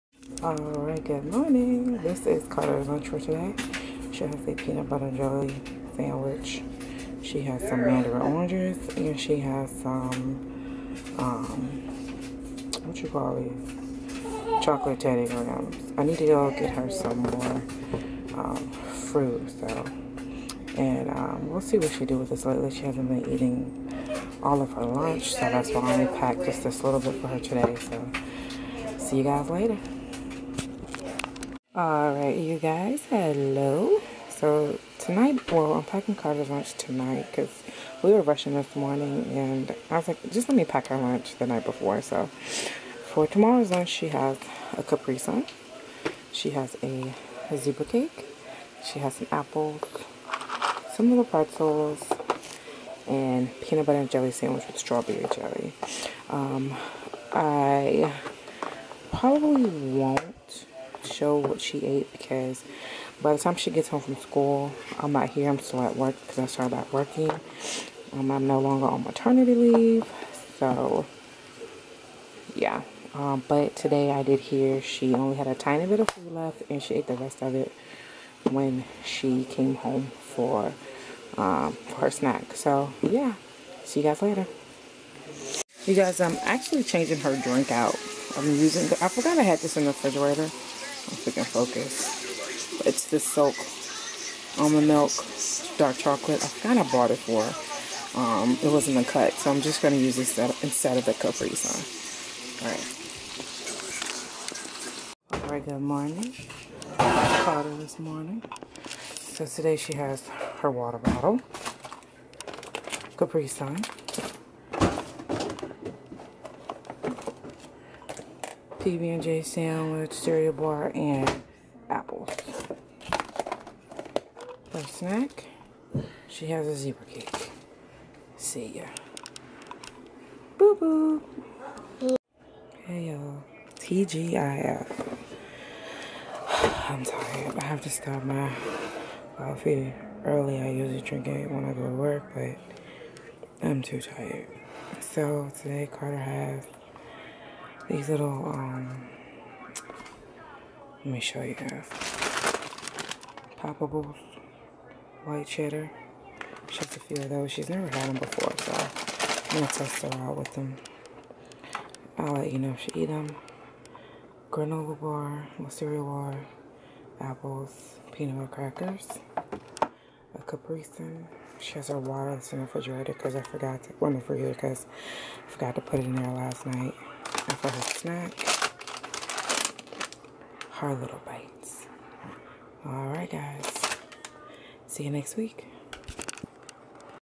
SCHOOL LUNCHES | SCHOOL LUNCH IDEA FOR PICKY EATER
Hey yall! So I forgot to record Mondays lunch. I have to remember to place the camera in the Kitchen, I hope you enjoys this week lunches. Also I started back back working this week so i did not get to record what she ate by the time i come home she has had her left over lunch for her snack. Hope yall enjoy this video.